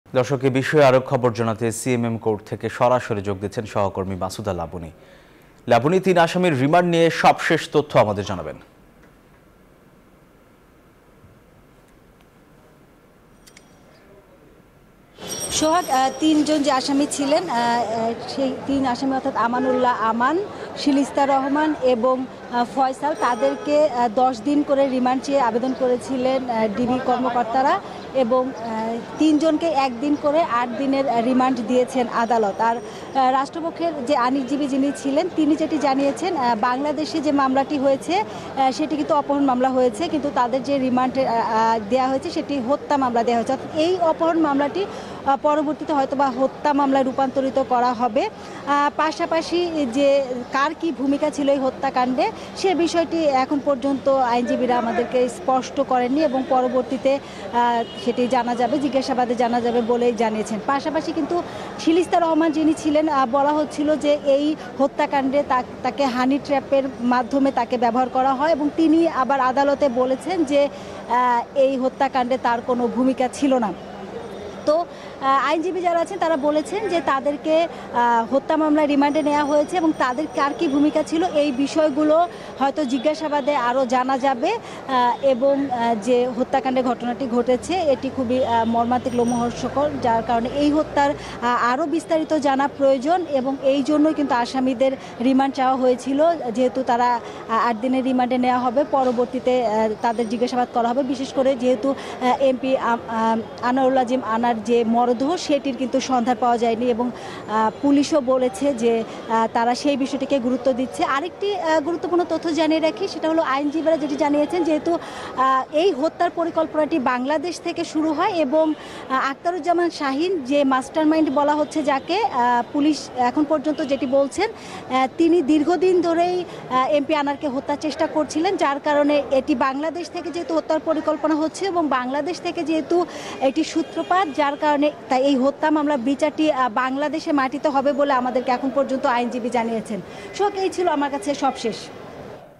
এমপি আনার হত্যাকাণ্ডের খবর জানতে সিএমএম কোর্ট থেকে সরাসরি | News24
NEWS24 Team